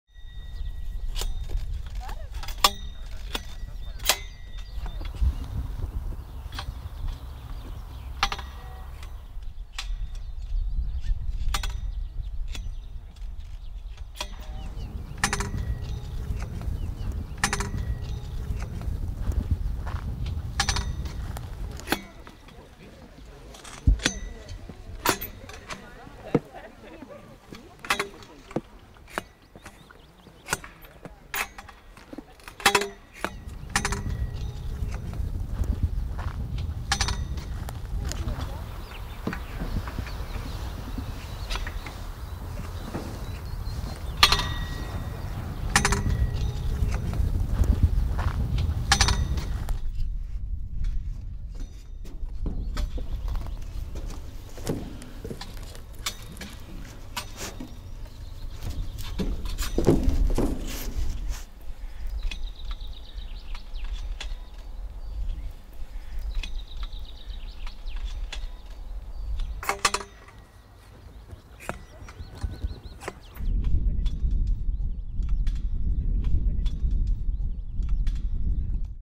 Лукашенко во время субботника посадил деревья в Национальном парке "Припятский"
Президент Беларуси Александр Лукашенко сегодня принял участие в республиканском субботнике, высадив деревья на территории Национального парка "Припятский".